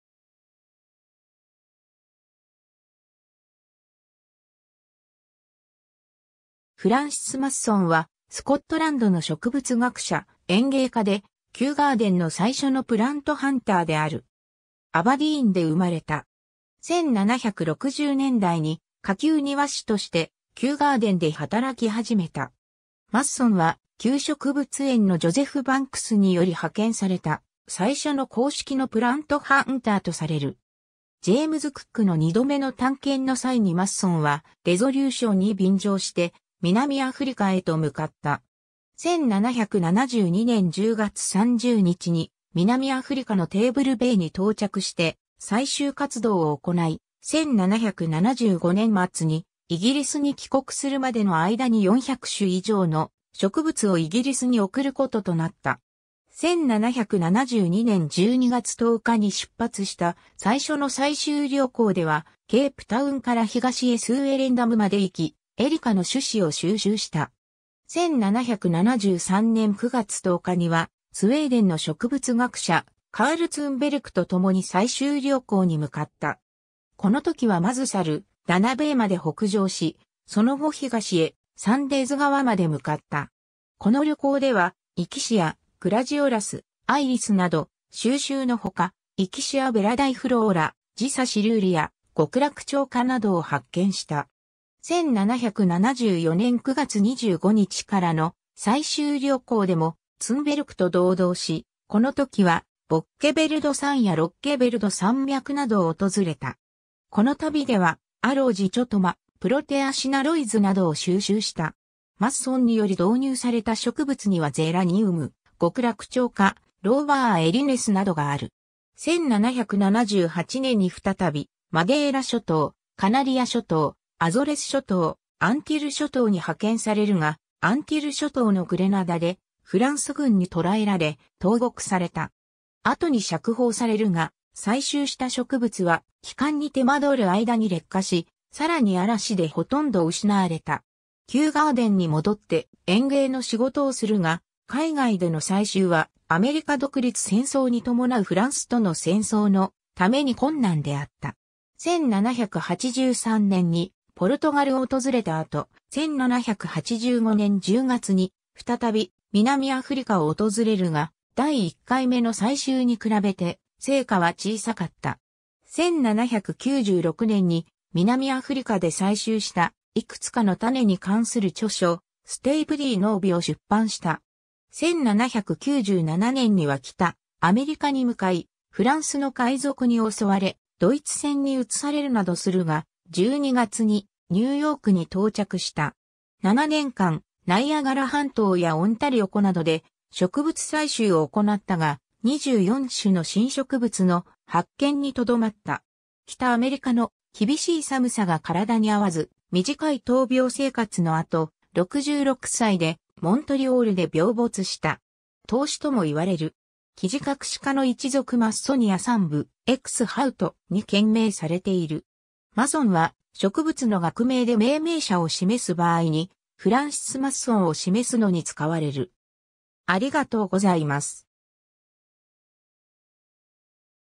フランシス・マッソン
フランシス・マッソン（Francis Masson、1741年8月 - 1805年12月23日）はスコットランドの植物学者、園芸家で、キューガーデンの最初のプラントハンター（植物収集の専門家）である。
 アバディーンで生まれた。
 1760年代に、下級庭師としてキューガーデンで働き始めた。
 マッソンはキュー植物園のジョゼフ・バンクスにより派遣された最初の公式のプラントハンターとされる。
ジェームズ・クックの2度目の探検の際にマッソンは「レゾリューション」に便乗して南アフリカへと向かった。
1772年10月30日に南アフリカのテーブル・ベイに到着して採集活動を行い、1775年末にイギリスに帰国するまでの間に400種以上の植物をイギリスに送ることとなった。
1772年12月10日に出発した最初の採集旅行ではケープタウンから東へスウェレンダムまで行き、エリカの種子を収集した。
1773年9月10日にはスウェーデンの植物学者カール・ツンベルクとともに採集旅行に向かった。
この時はまずサルダナ・ベイまで北上し、その後東へサンデーズ川まで向かった。
この旅行ではイキシア、グラジオラス、アイリスなど収集の他、Ixia viridiflora、Disa caerulea、ゴクラクチョウカ (Strelitzia reginae) などを発見した。
1774年9月25日からの採集旅行でもツンベルクと同道し、この時はボッケヴェルド山やロッゲヴェルド山脈などを訪れた。
この旅ではAloe dichotoma、Protea cynaroidesなどを収集した。
マッソンにより導入された植物にはゼラニウム、ゴクラクチョウカ、Loberia erinusなどがある。
 1778年に再び、マデイラ諸島、カナリア諸島、アゾレス諸島、アンティル諸島に派遣されるが、アンティル諸島のグレナダで、フランス軍に捕らえられ、投獄された。
後に釈放されるが、採集した植物は帰還に手間取る間に劣化し、さらに嵐で殆ど失われた。
キューガーデンに戻って、園芸の仕事をするが、海外での採集はアメリカ独立戦争に伴うフランスとの戦争のために困難であった。
 1783年にポルトガルを訪れた後、1785年10月に再び、南アフリカを訪れるが、第1回目の採集に比べて、成果は小さかった。
1796年に、南アフリカで採集したいくつかの種に関する著書、"Stapeliae Novae"を出版した。
、1797年には北アメリカに向かい、フランスの海賊に襲われ、ドイツ船に移されるなどするが、12月にニューヨークに到着した。
7年間、ナイアガラ半島やオンタリオ湖などで植物採集をおこなったが、24種の新植物の発見にとどまった。
北アメリカの厳しい寒さが身体に合わず、短い闘病生活のあと、66歳でモントリオールで病没した。
凍死ともいわれる。
 キジカクシ科の1属 Massonia Thunb. ex Houtt.に献名されている。
 Massonは、植物の学名で命名者を示す場合にフランシス・マッソンを示すのに使われる。